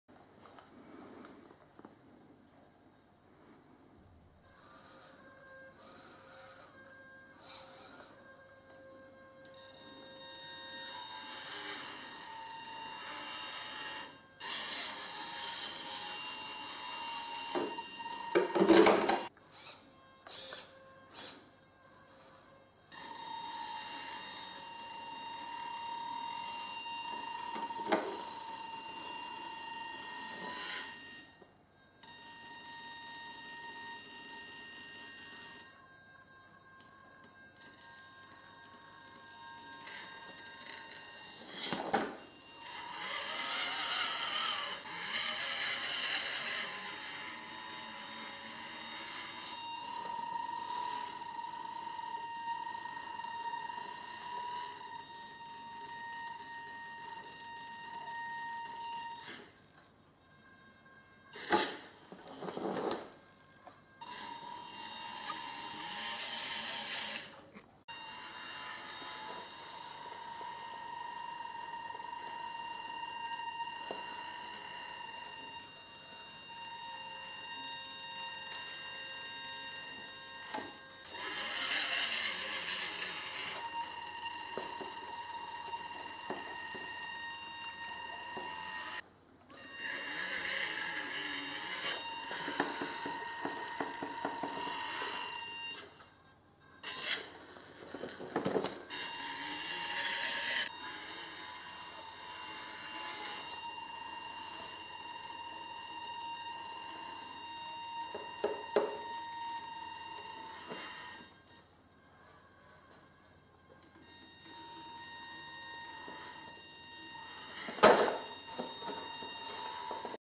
Tamiya CR-01 climbing up the wall
Climbing up the wall with my Tamiya CR-01 Stadium Blitzer